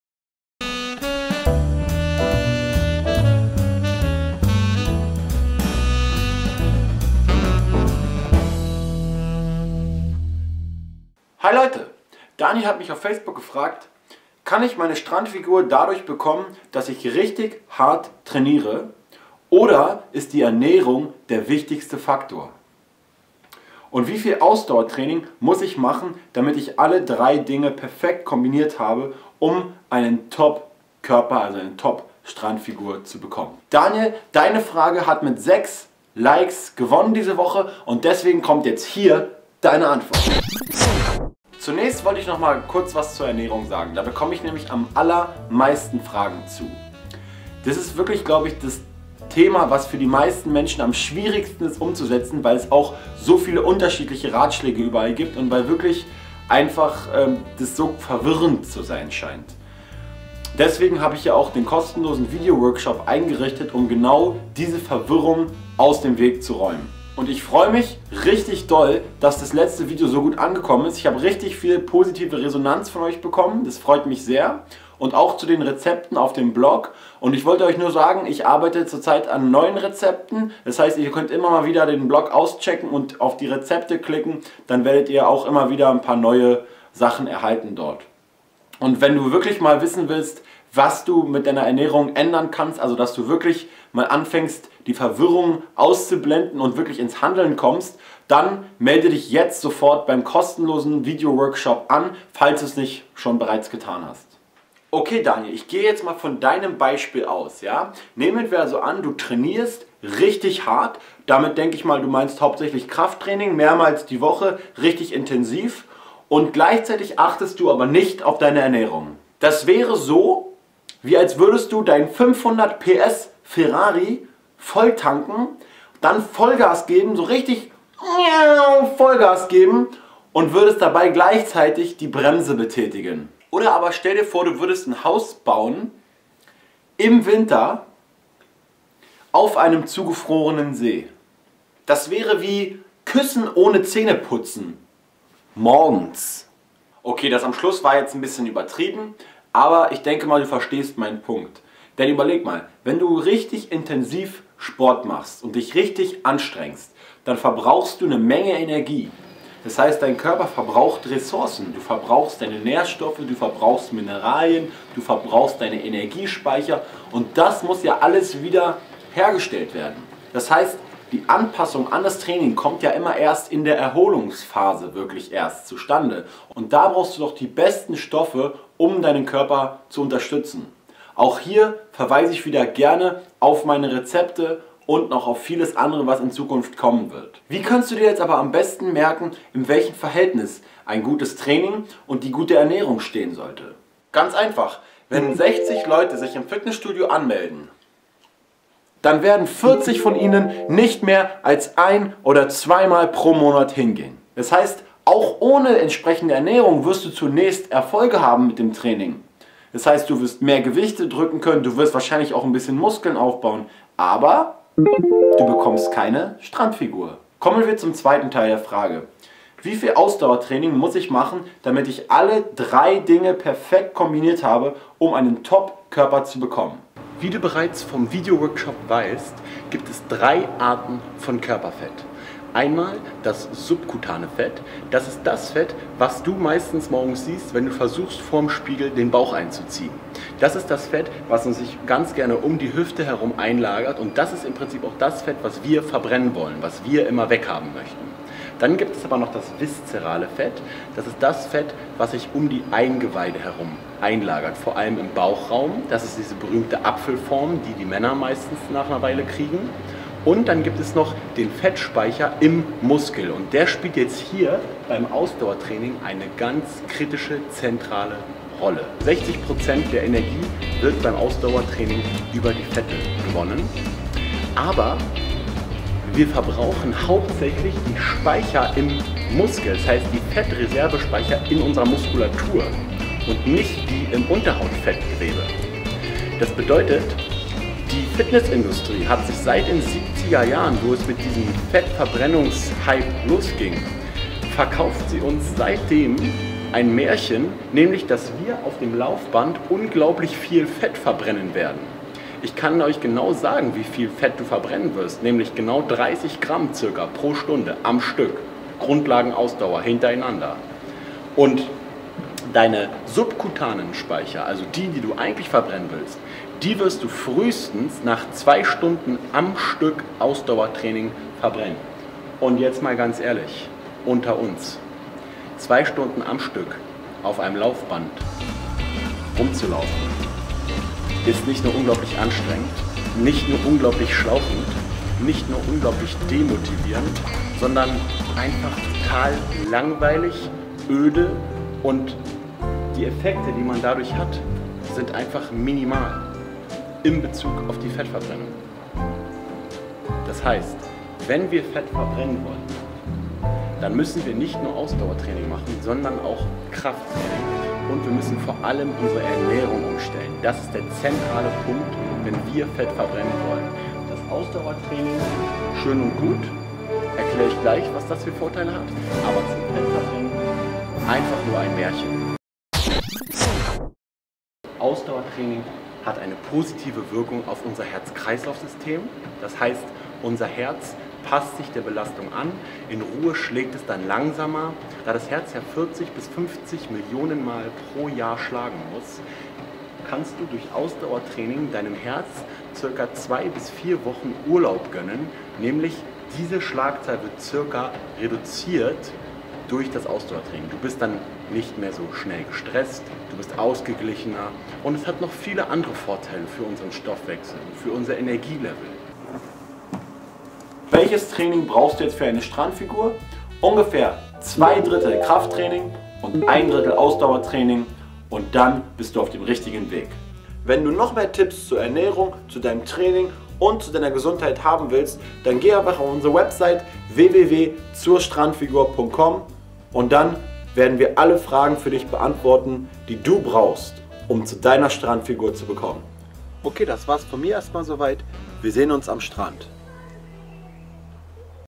WELCHES Training zum Fettabbau? Trainiere deine KRAFT
trägst du dich zum kostenlosen Abnehm-Videokurs ein.

Daniel hat auf Facebook gefragt: Kann ich meine Strandfigur dadurch bekommen, dass ich richtig hart trainiere, oder ist die Ernährung der wichtigste Faktor? Und wie viel Ausdauertraining muss ich machen, damit ich alle 3 Dinge perfekt kombiniert habe, um einen top-Körper zu bekommen...